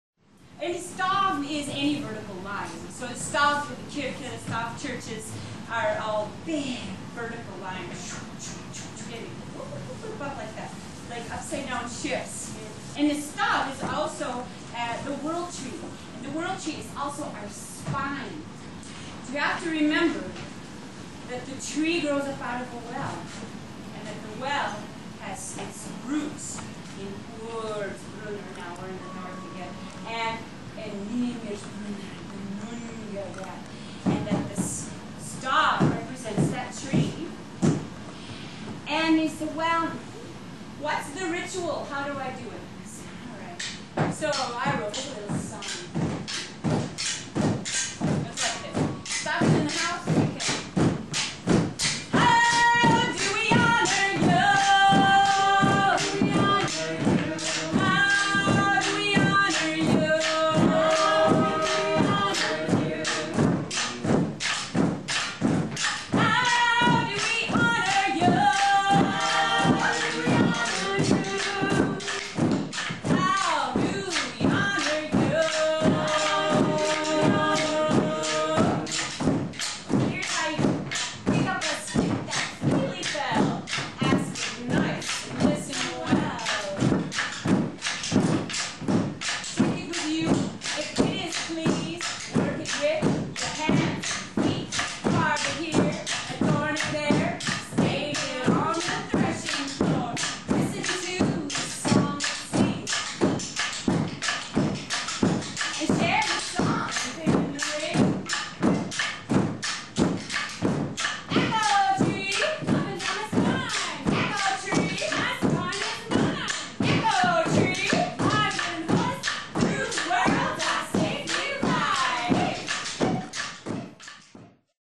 Volva Stav - the staff carrier
Stav is the world tree, our walking stick, axis mundi, our spine.  Performances at the Bryant Lake Bowl Theater and Cedar Cultural Center combine to teach about the staff carrier of Norse tradition.
Video editing John Faeth 2008